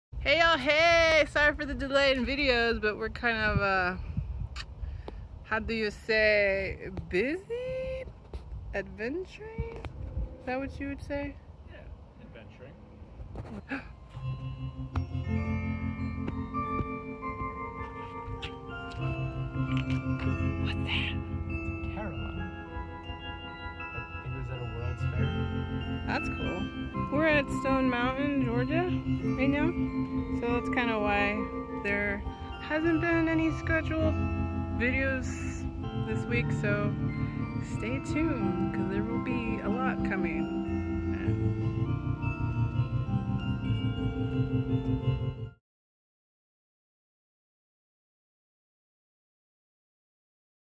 Sorry For The Delay // Channel Update
Just a little friendly update apologizing for the delay on new videos! We've been out adventuring and trying to gather some enjoyable content to share with all of you!

Please be patient, I'm learning and will have loads of videos coming at you real soon!

-Amber